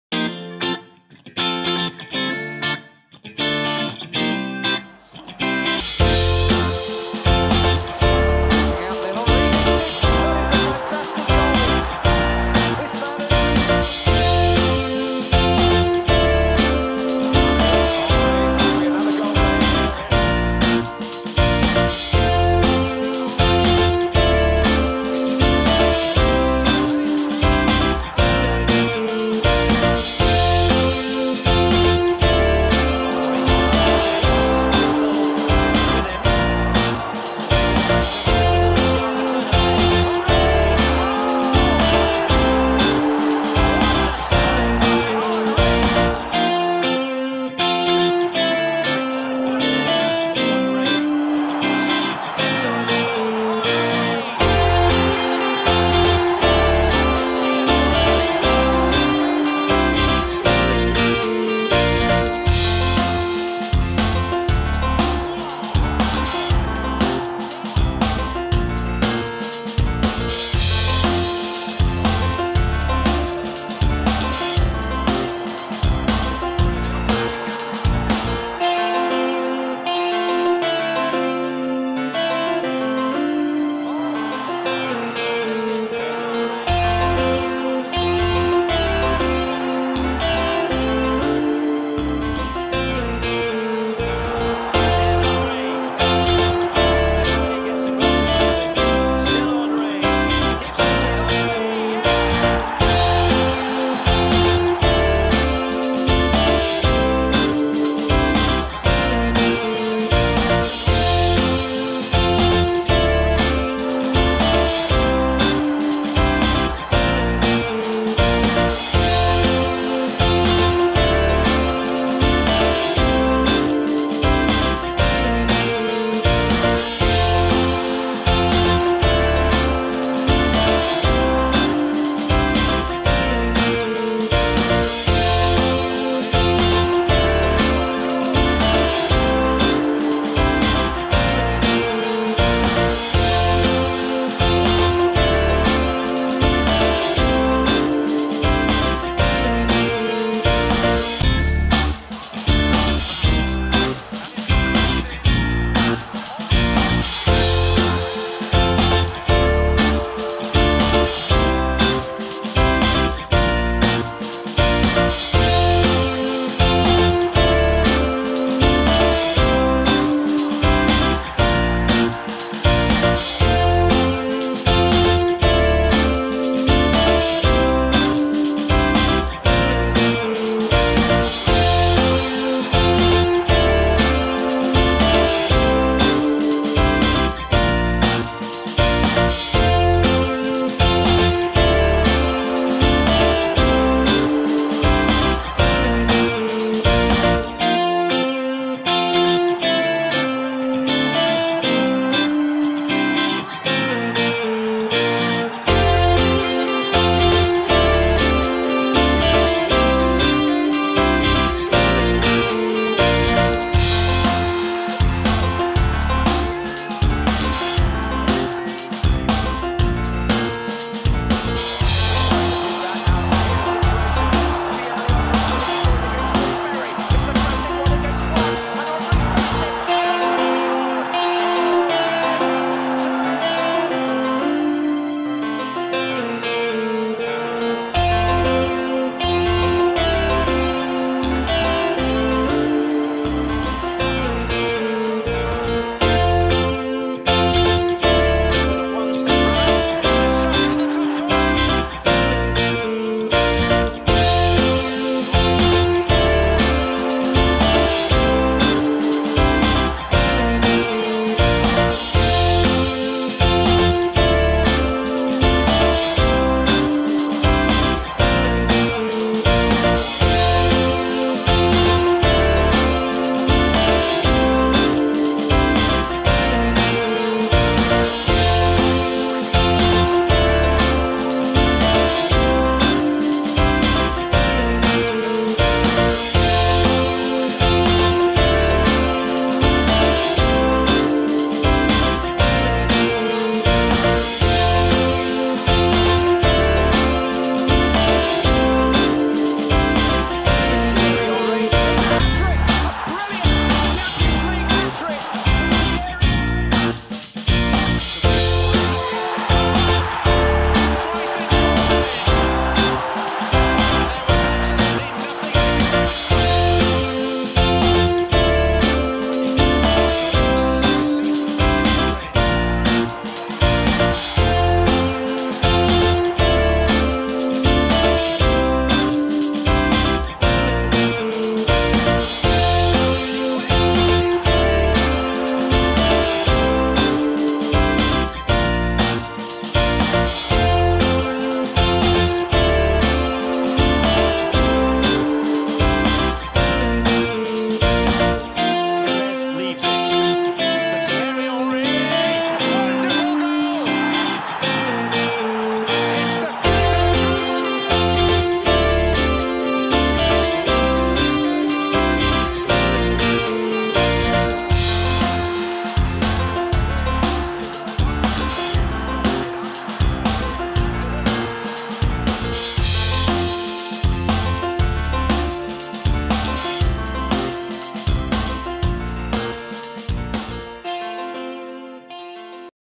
Defenders Nightmares: Thierry Henry
Premier League Hall of Fame - home of the greats.

Pace, power and grace. Thierry Henry gave Premier League defenders nightmares for eight seasons. He scored over 20 Premier League goals five seasons running, claiming four Golden Boots, with some outrageous strikes. Not content with just scoring he also provided 74 assists, including a record 20 in 2002/03, won two Player of the Season awards and two Premier League titles.